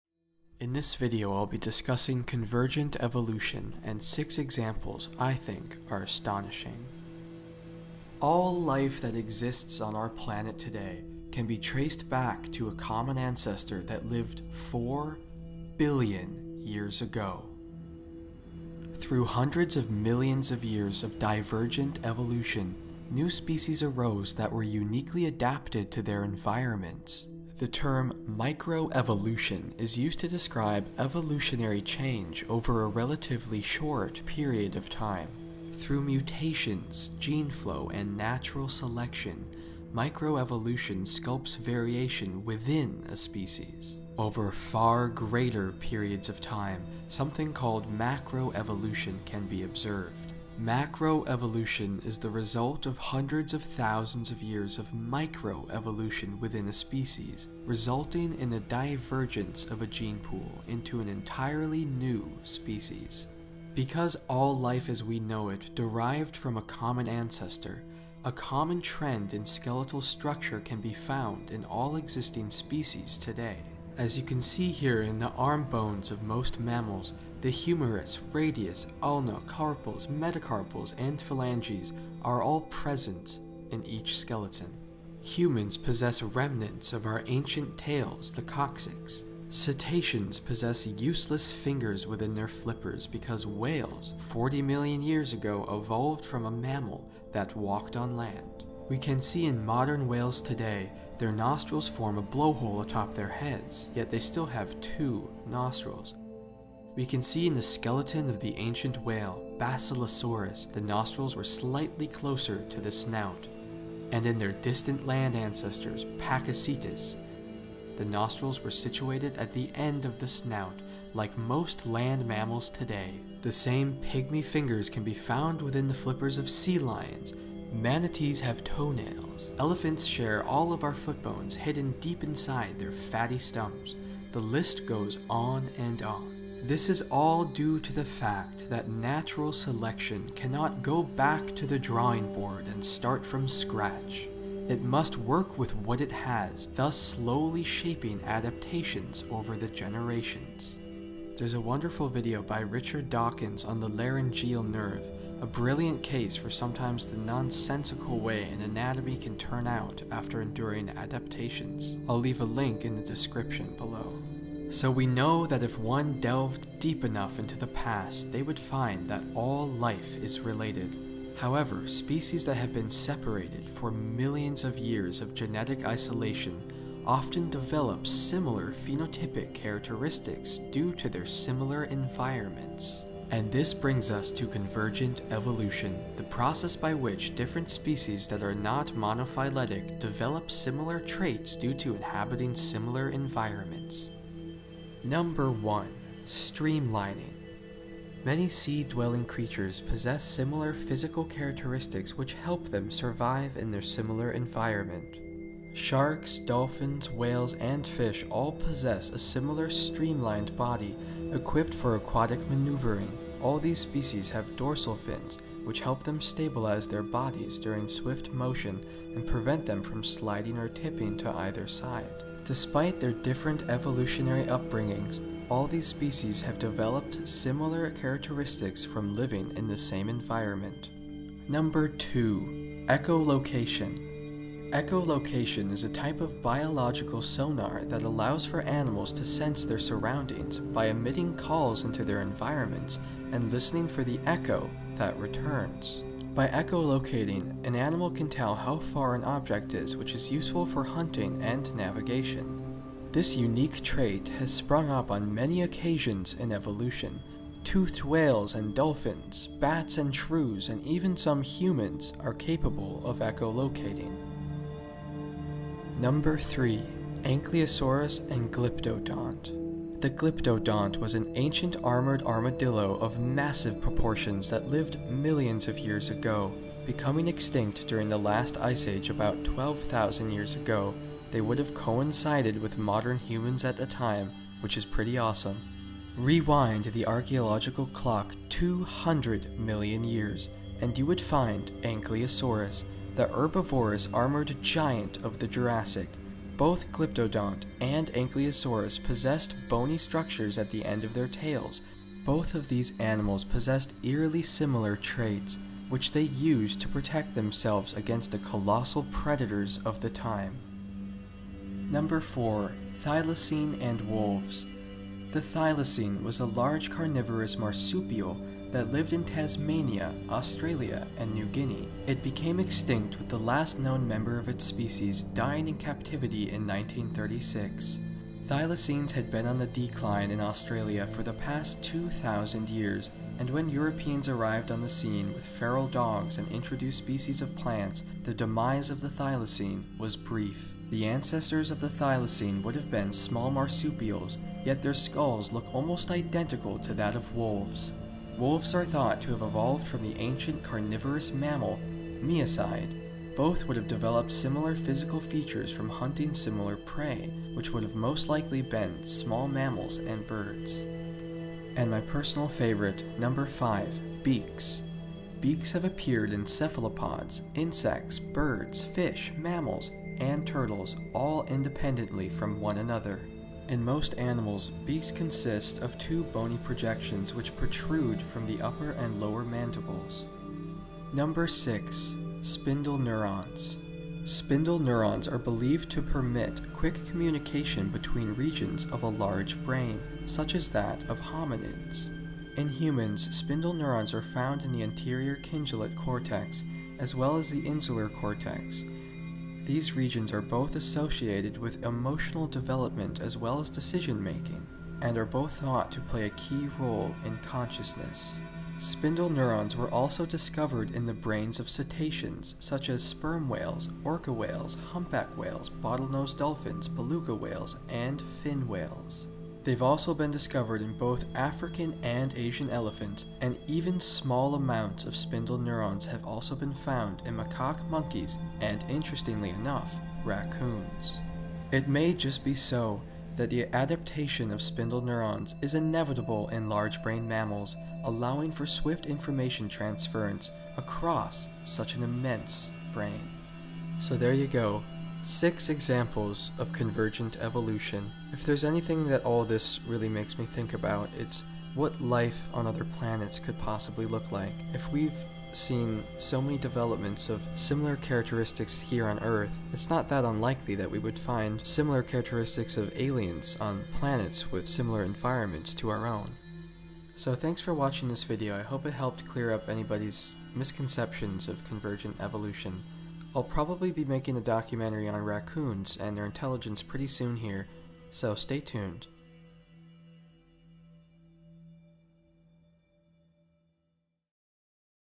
Top 6 Examples of Convergent Evolution
In this video I briefly discuss evolution before moving on to convergent evolution and 6 of my favorite examples.

The cartoons and animations were done by me. Most of the pictures were taken from Unsplash and Pexels.

by Shadows On The Snow

Created by John Frenzel